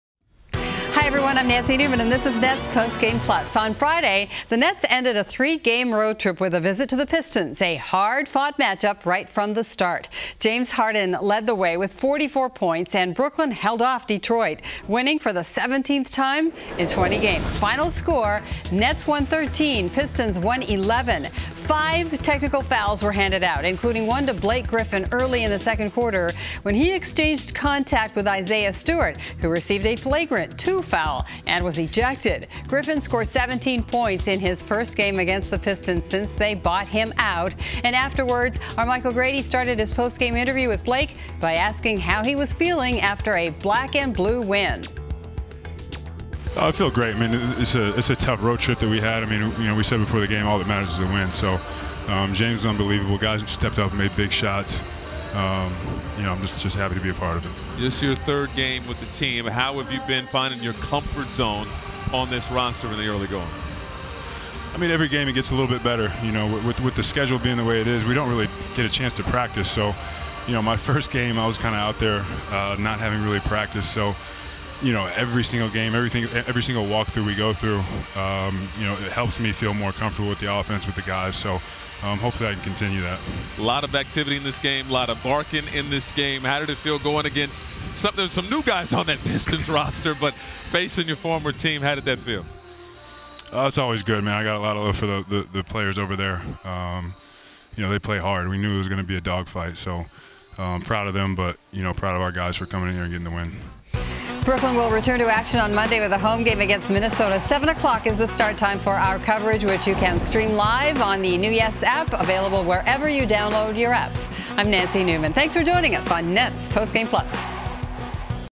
Nets PGP: Nets beat the Pistons, 113-111
James Harden's 44 points led the way to a Nets 113-111 victory over the Pistons.